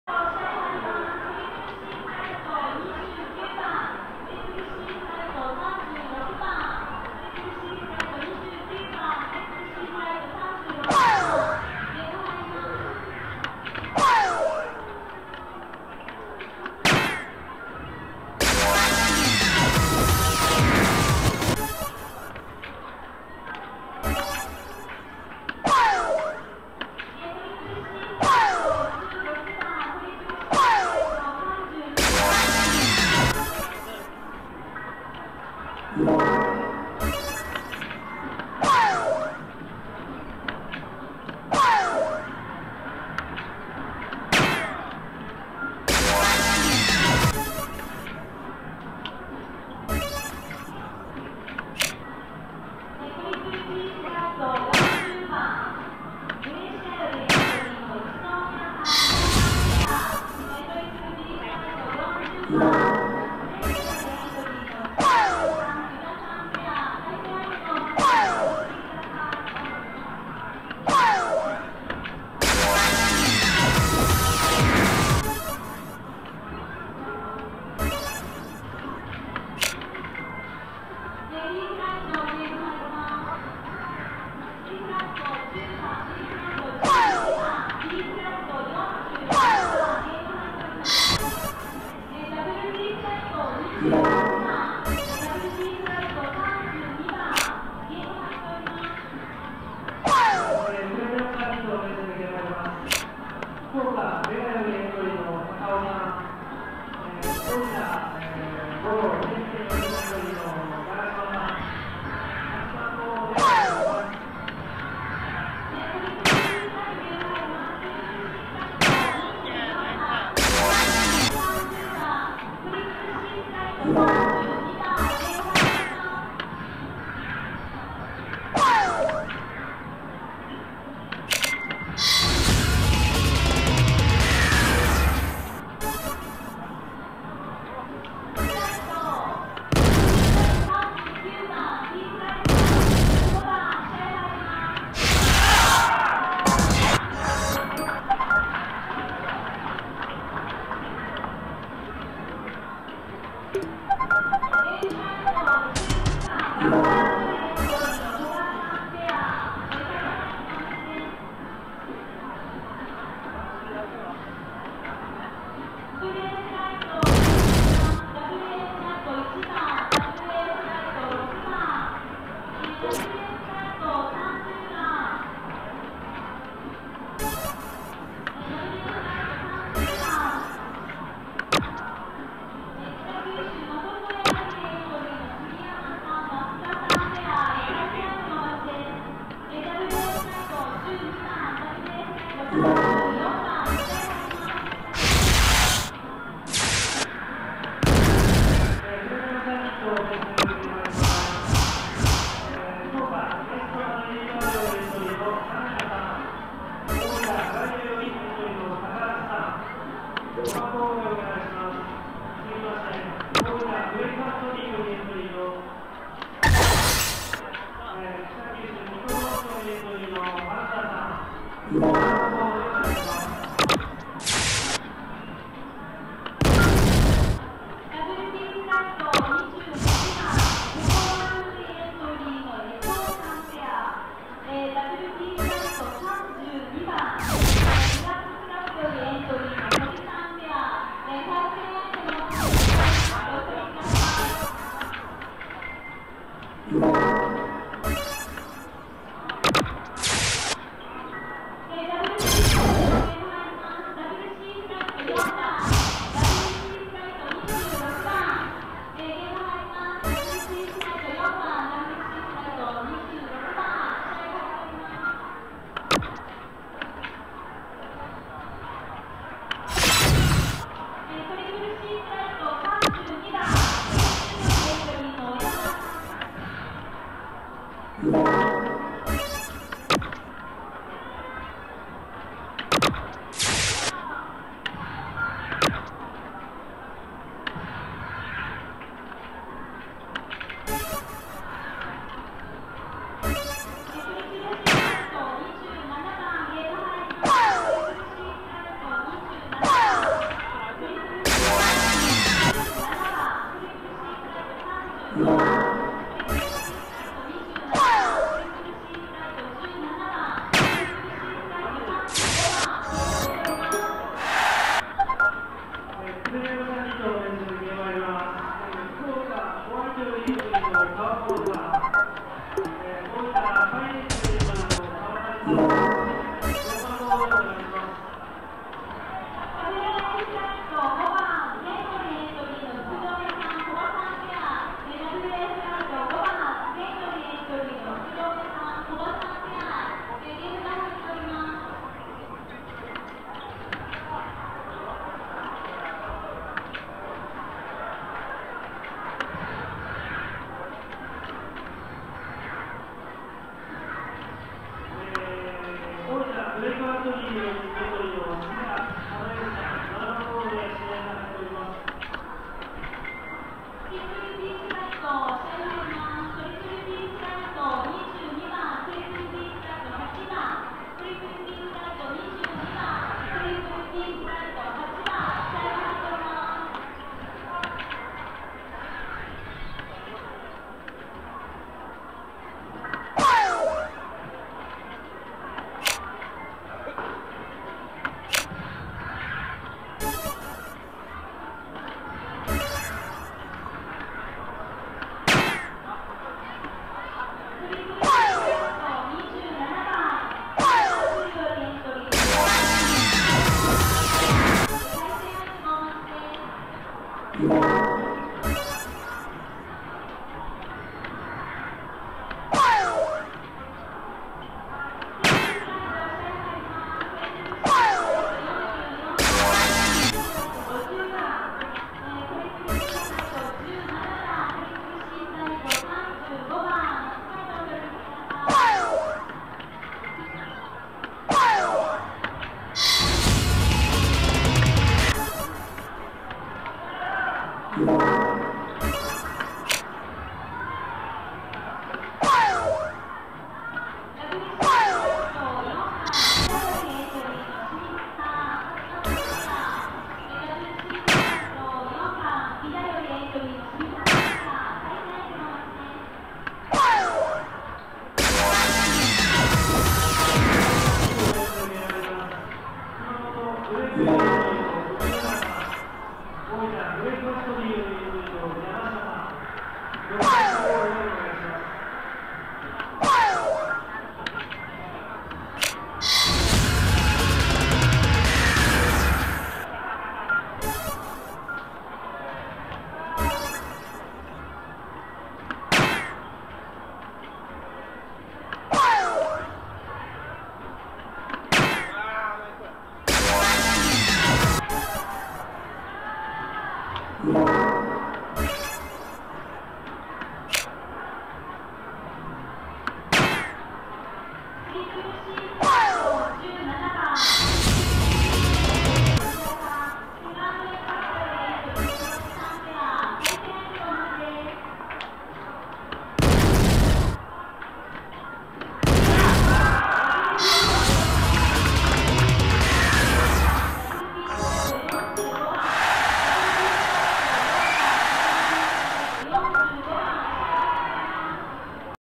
【大分大会2011】西・山田ペアvs末吉・梶原ペア 【MAXフライト決勝T】
PHOENIX CUP in 大分 2011.7.3　大分イベントホール　MAXフライト決勝トーナメント
西・山田ペアvs末吉・梶原ペア